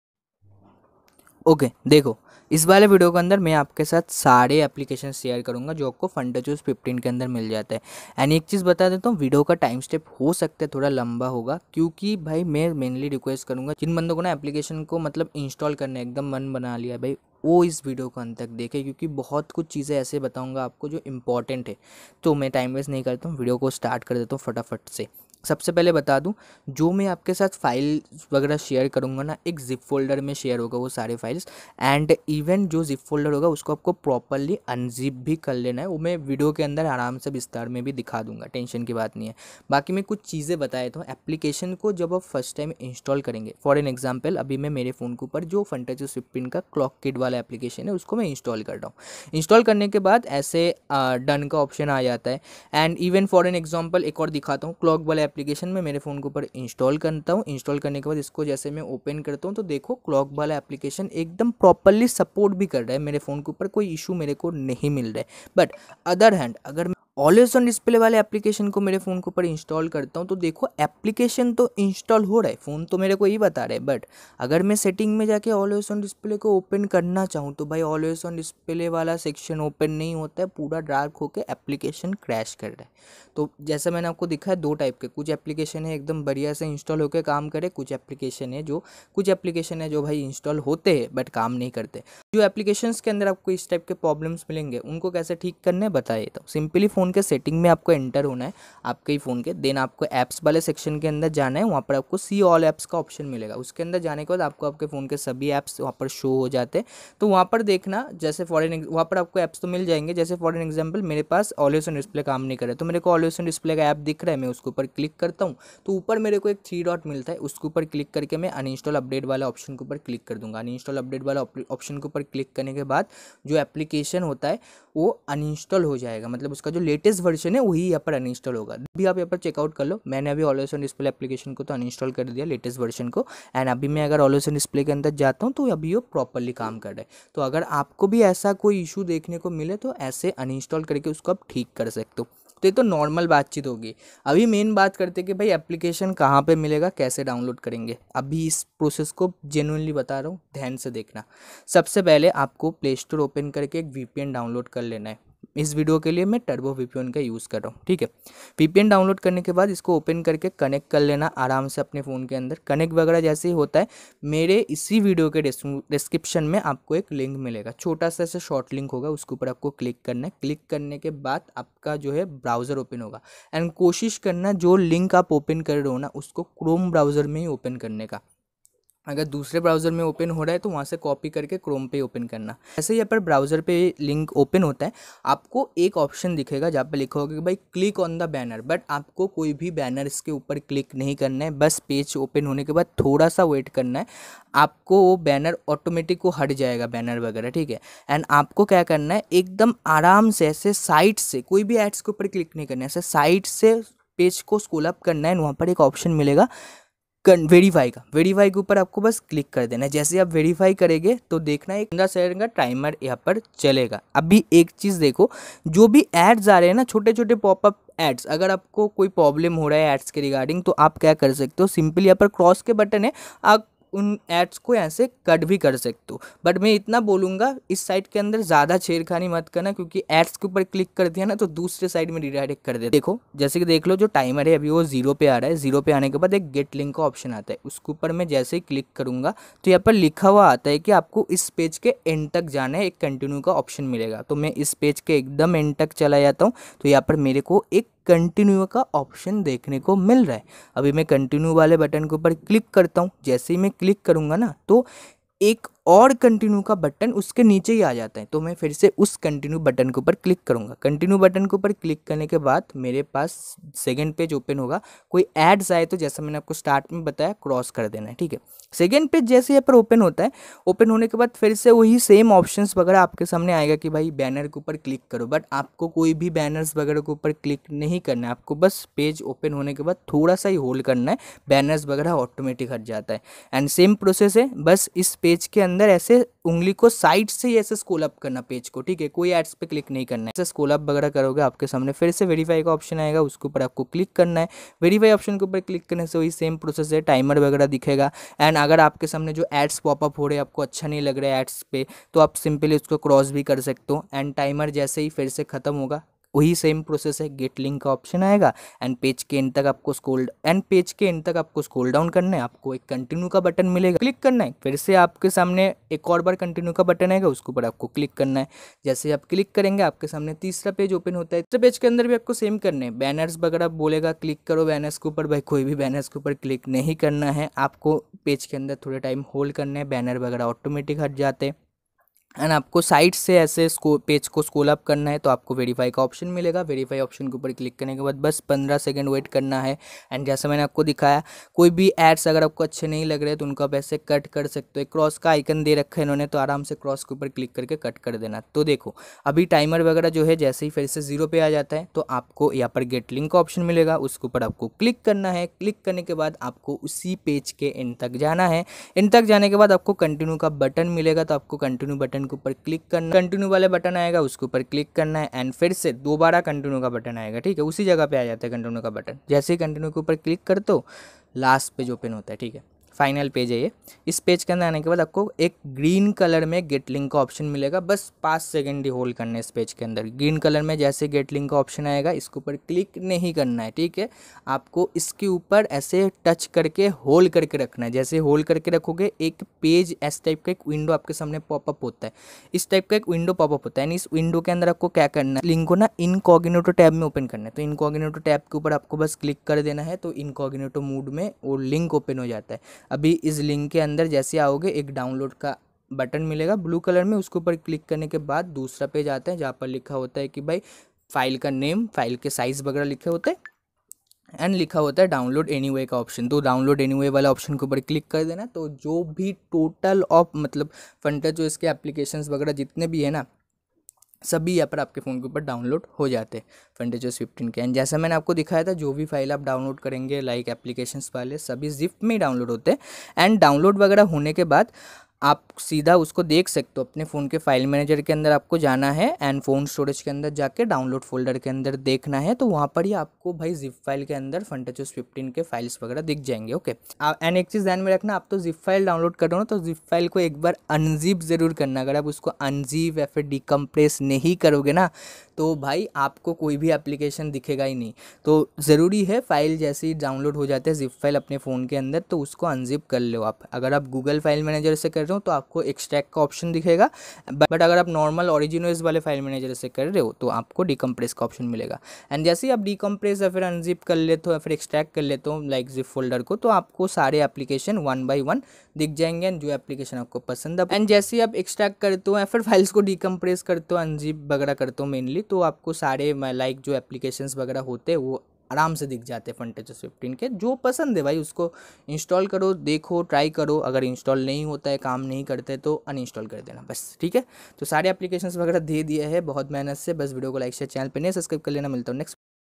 Funtouch Os 15 All Apks Download | Funtouch Os 15 All Apps | IQoo 12 Funtouch Os 15 Apks
Notice 🚫 🚫 🚫 The Download Process Are Change Check This 1 Min Video

MY GEAR USED FOR YOUTUBE VIDEOS :

👉 Green Screen

ABOUT THIS VIDEO :
I Told In This Video About Funtouch Os 15 All Apps Download

QUERIES SOLVE IN THIS VIDEO:

Time Steps:

0:00 Introduction
0:26 File Zip
0:42 Funtouch Os 15 All Apps Install Process
2:24 Funtouch Os 15 Apke Download Process